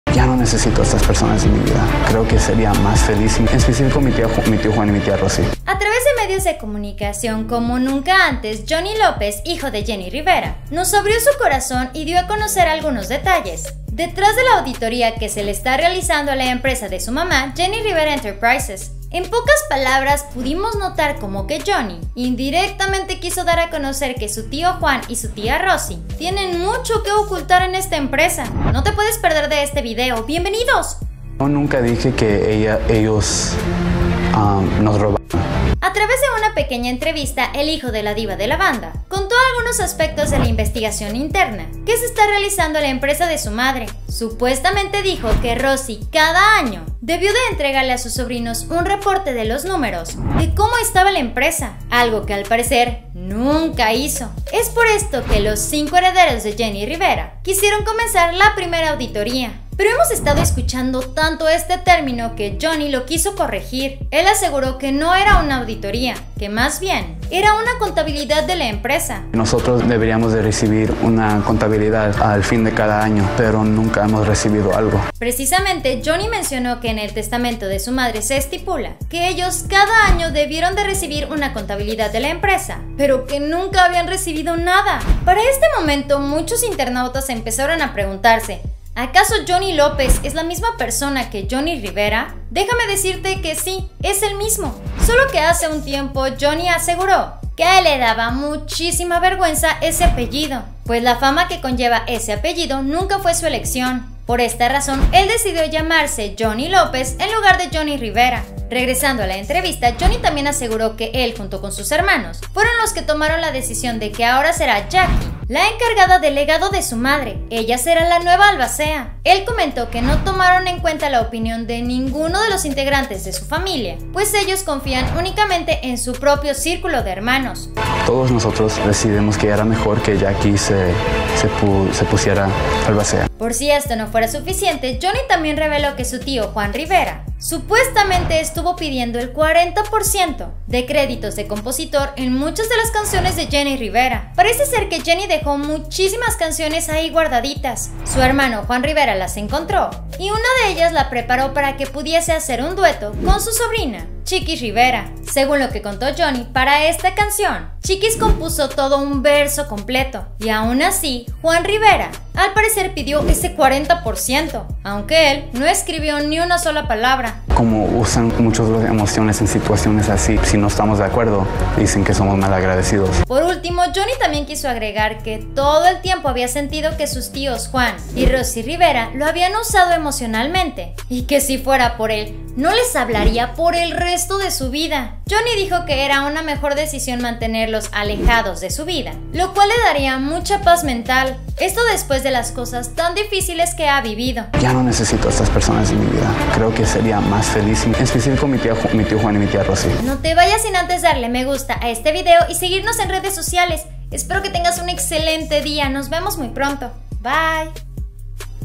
🔴😭Ultimo Minuto 𝗦𝗘 𝗚𝗔𝗦𝗧𝗔𝗥𝗢𝗡 𝗘𝗟 𝗗𝗜𝗡𝗘𝗥𝗢 𝗤𝗨𝗘 𝗣𝗘𝗥𝗧𝗘𝗡𝗘𝗖𝗜𝗔 A Los hijos De Jenni Rivera 😱
Insolito lo que hicieron los tíos de Chiqui Rivera

¡ BIENVENIDOS ‼️